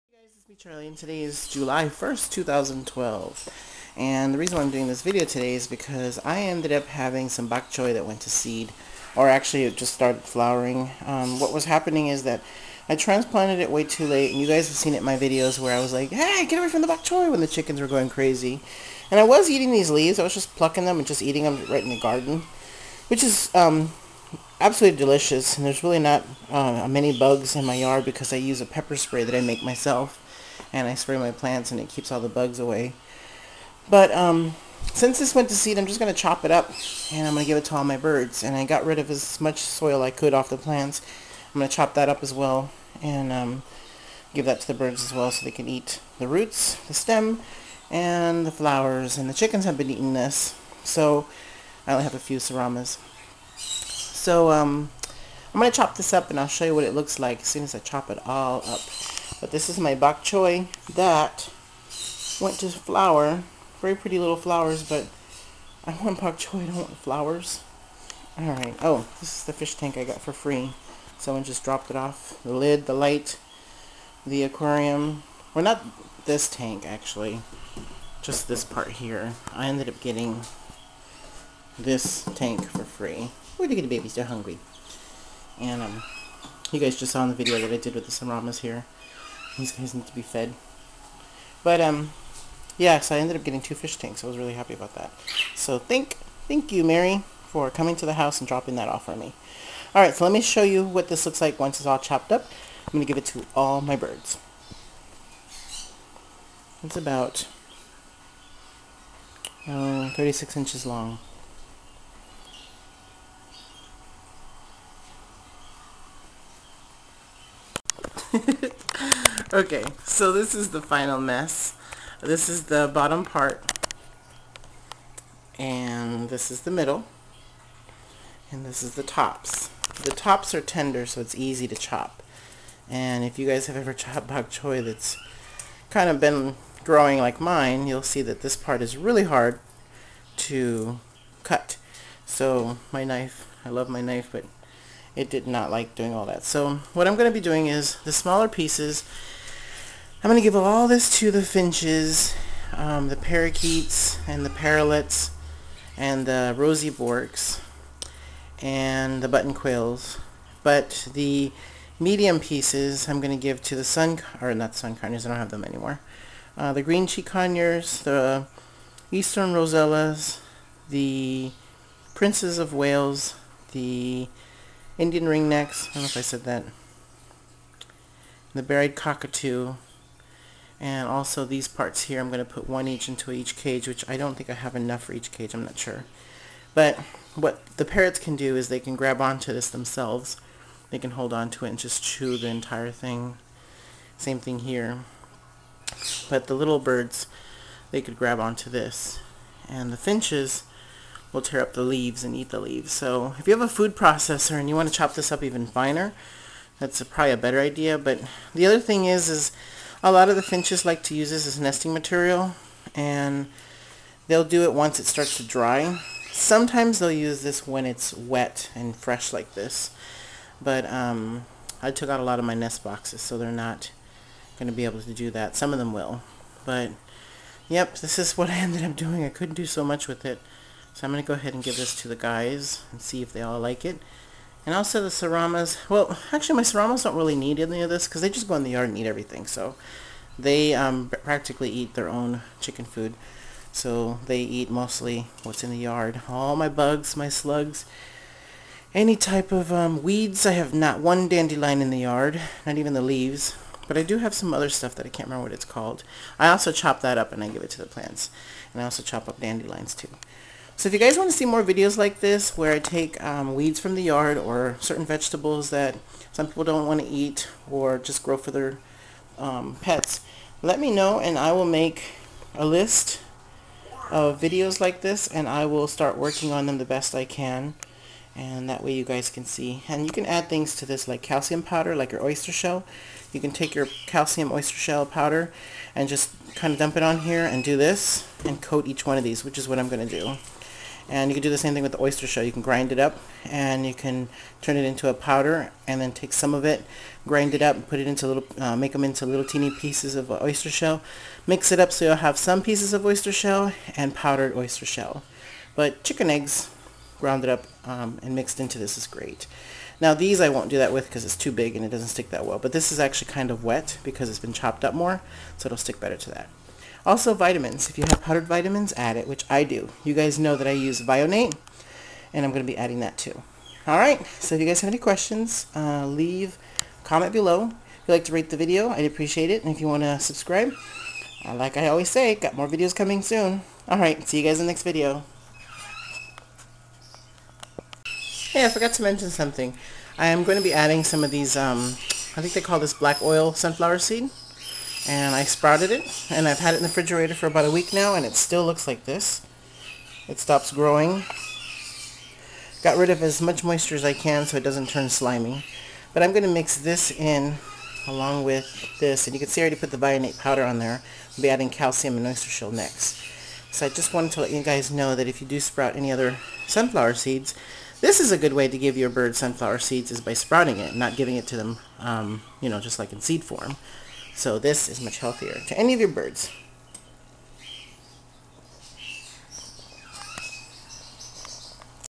Weeds From The Yard Turned In To Feed For Your Birds
Make sure you google the right types of weeds from the yard before giving to any pets. also on youtube I have seen videos on the right kinds of weeds that are edible..